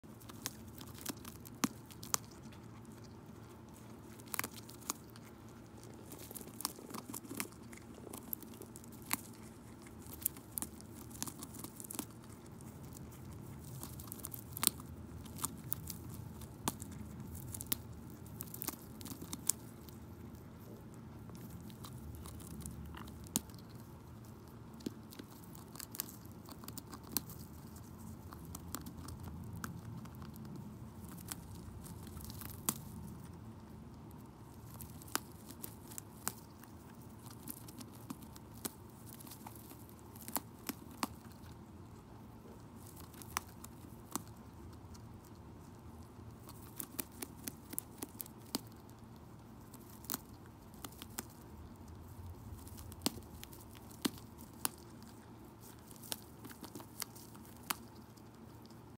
ガジガジコーンをがじがじするりちゃむ🐿リチャードソンジリス
ガジガジコーンをガジガジするお仕事です🌽✨✨